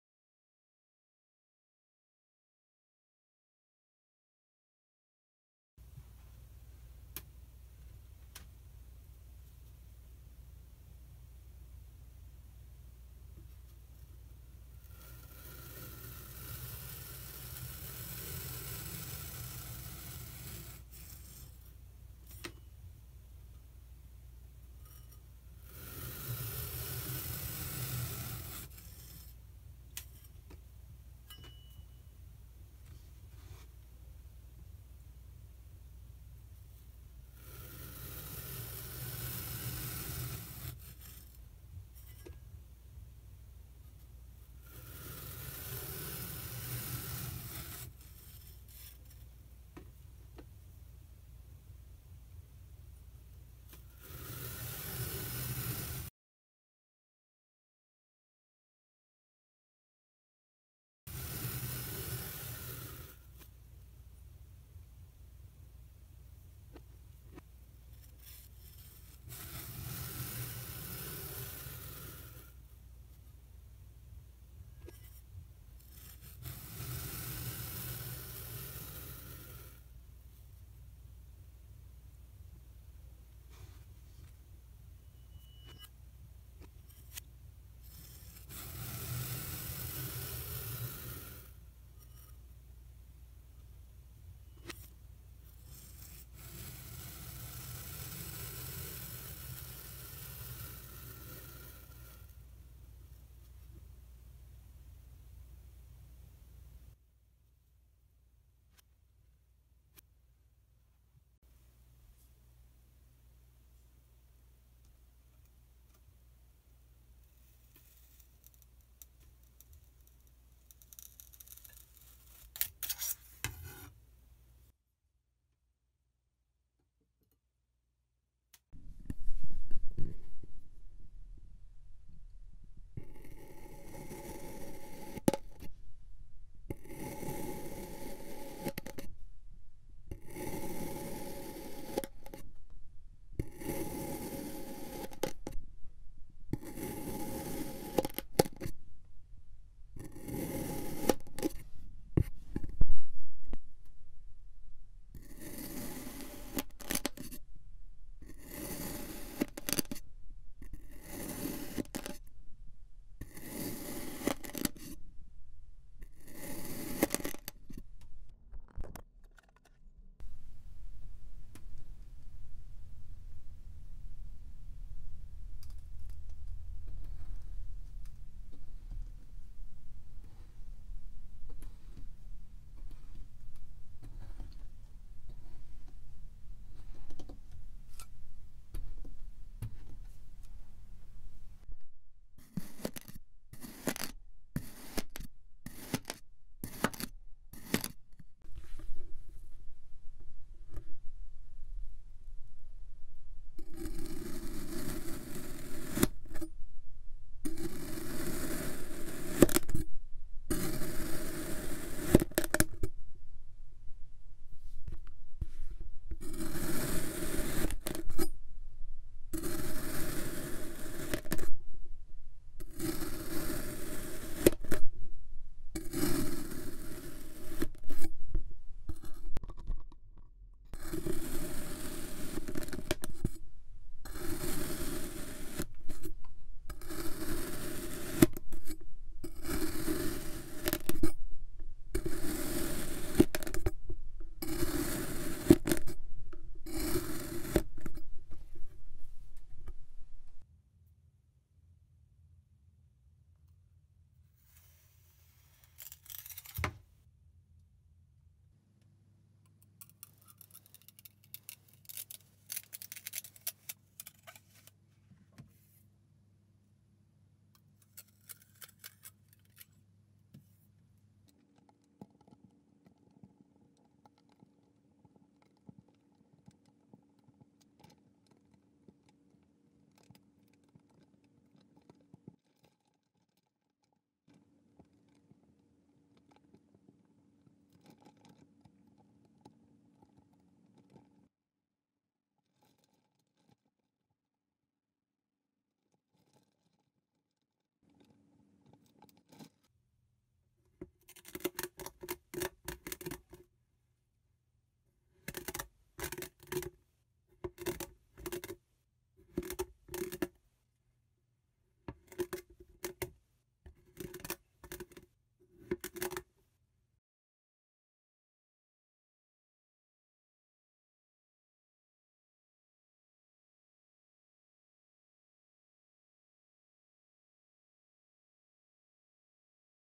Most Satisfying Video Ever ASMR #3 | Cheese Slicer vs Kinetic Sand Block | Sand Tagious
Oddly and Very Satisfying Kinetic Sand Video and ASMR Compilation #3. These videos were specifically chosen to help relax. In this video, we show clips from some of our best content for ASMR relaxation. Thanks!

Satisfying | Satisfying Video | Oddly Satisfying | The Most Satisfying | The Most Oddly Satisfying

Where can you get this sand?

You can purchase the same kinetic sand